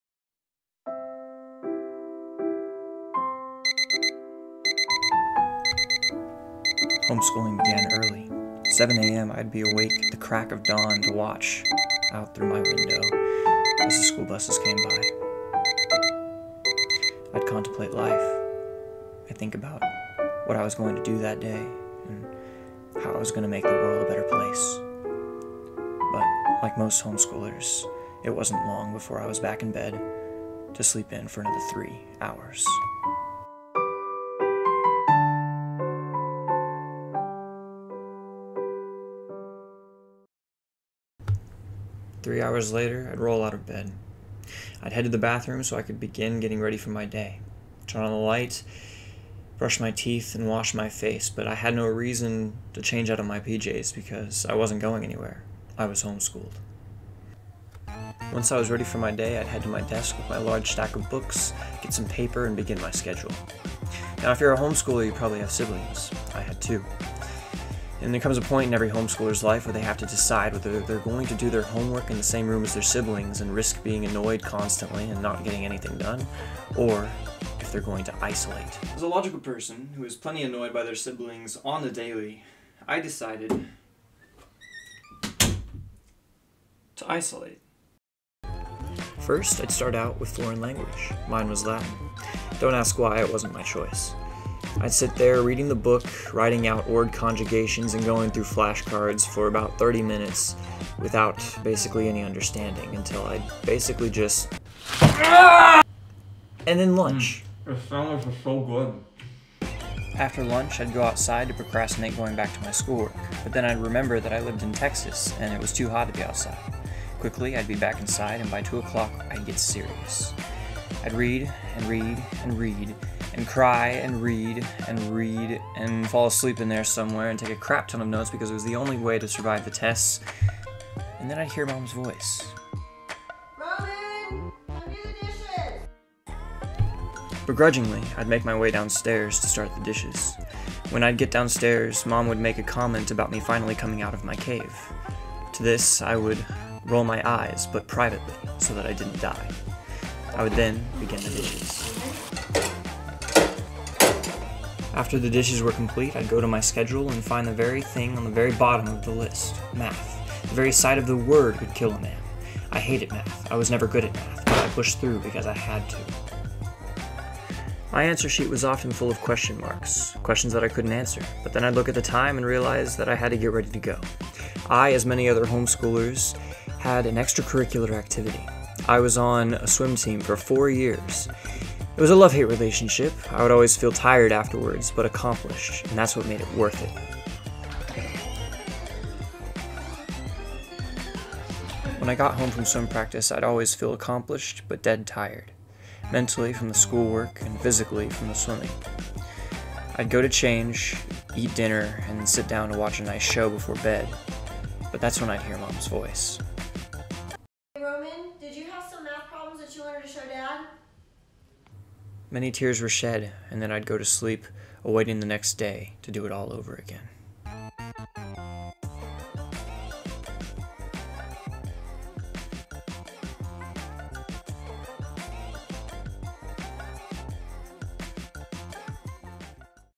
A day in the life of a homeschooler
What I did as a high schooled homeschooler, if you were wondering.